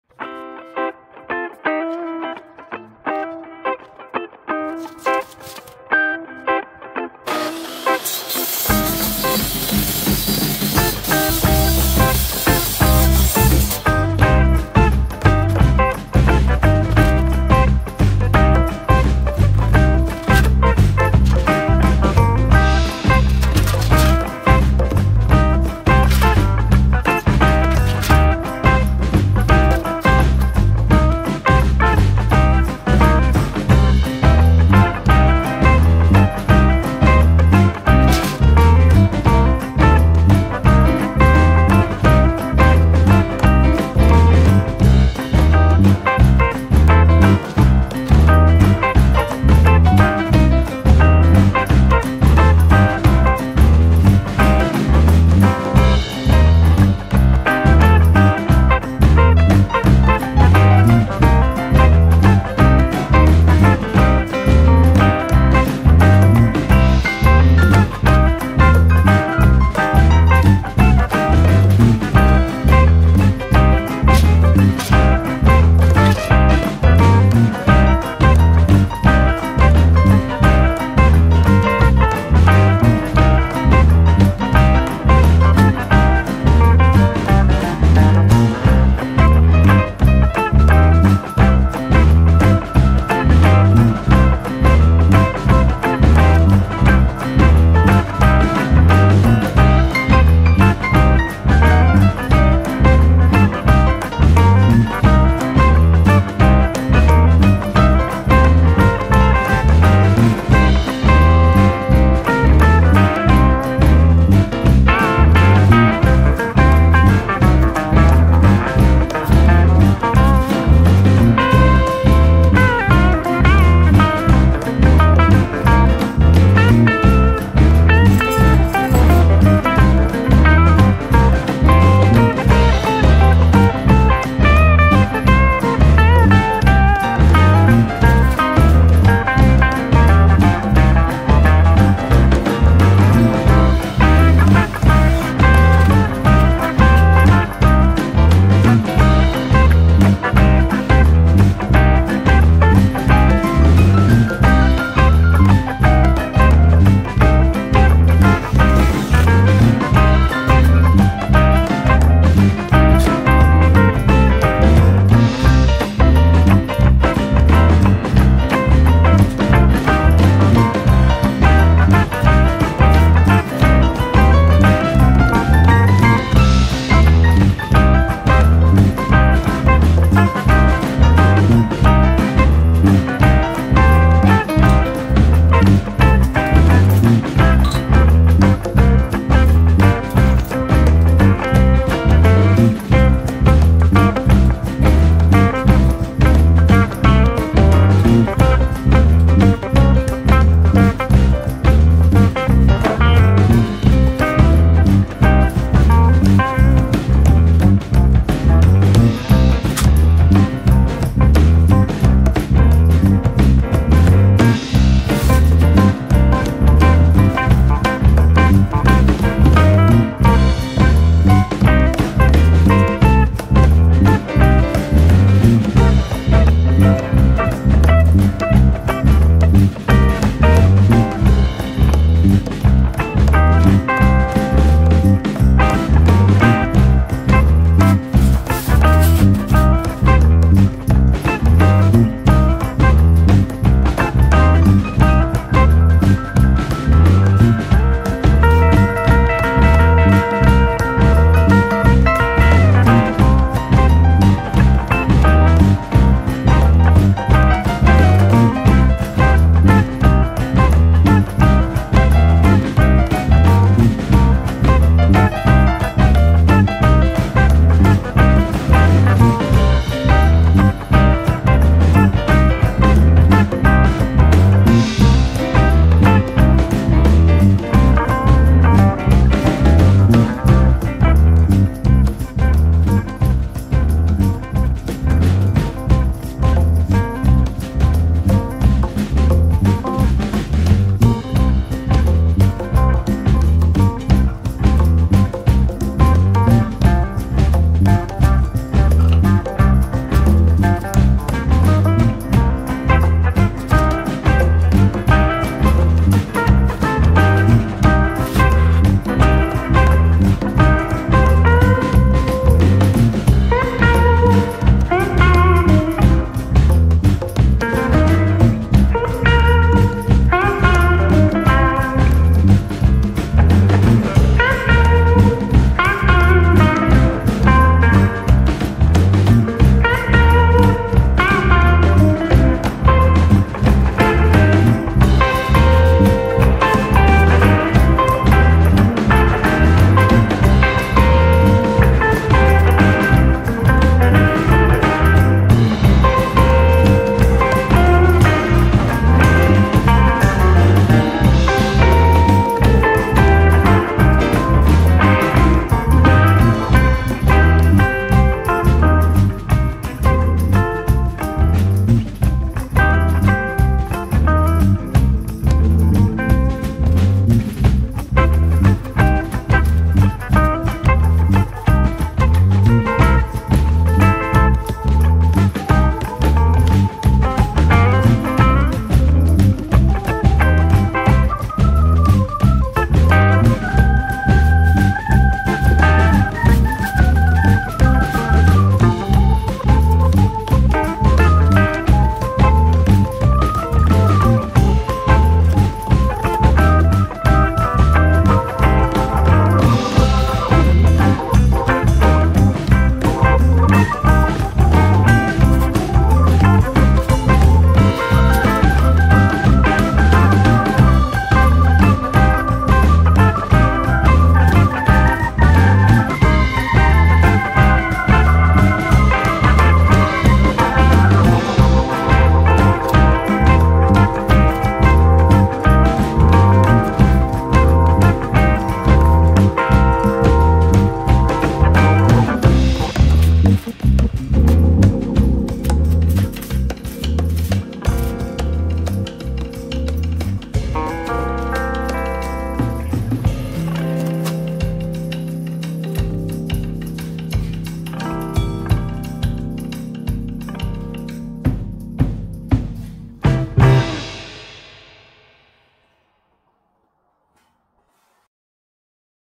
포토 스튜디오 뚝딱 만들기 Create a photo studio quickly 실용 인테리어
유리로 된 작업장이라 영상의 빛반사 문제를 해결하는 게 급했어요
잡동사니 창고를 뒤져서 있는 재료로 잠깐 동안 만들어 봤네요
Since the workshop is made of glass, it was urgent to solve the video's light reflection problem. I searched through the miscellaneous warehouse and made it for a while using materials I had available.